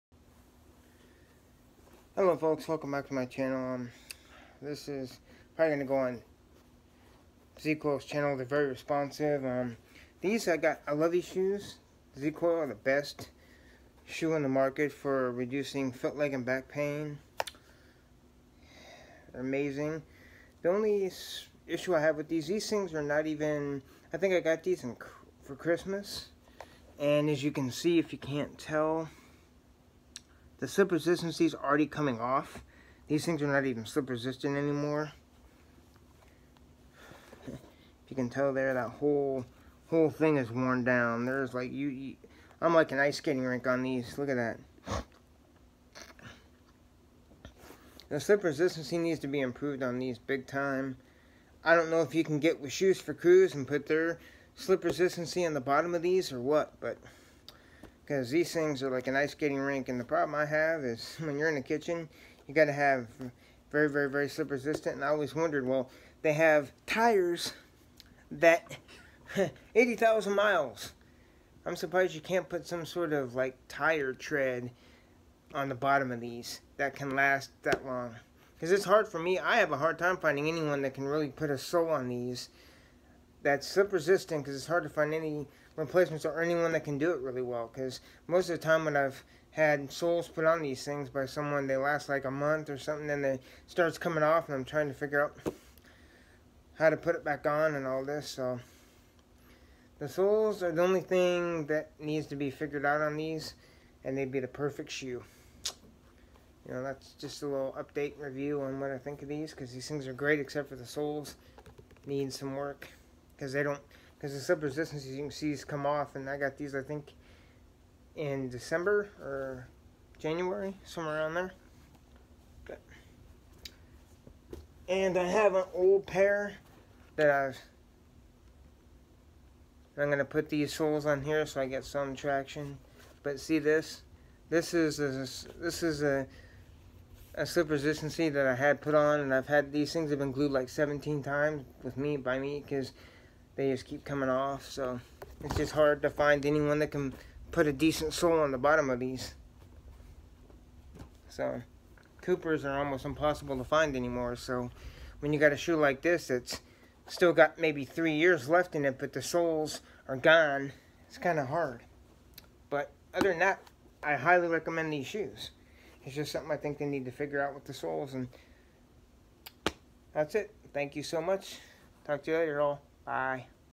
Z-coil shoes are great, but slip resistant soles are not that great!
This is a review and my thoughts on the slip resistant soles on the z-coil pain relief footwear.